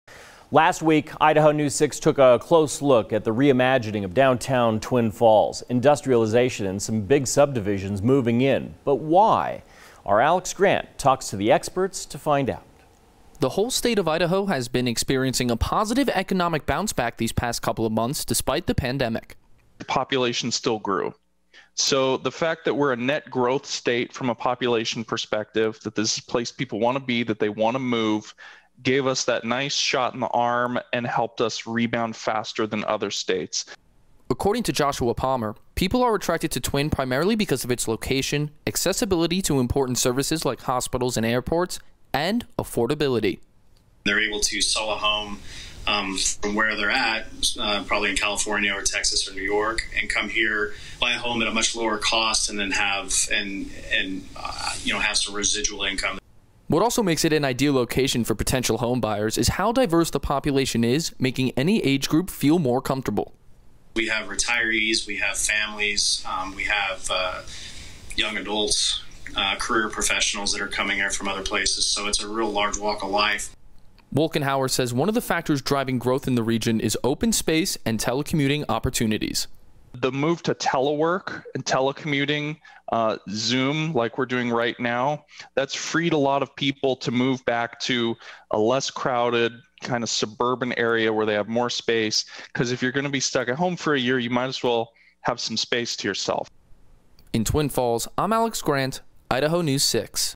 Twin Falls Growth
The increasing interest to the area is attributed to its location, accessibility to essential services and affordability.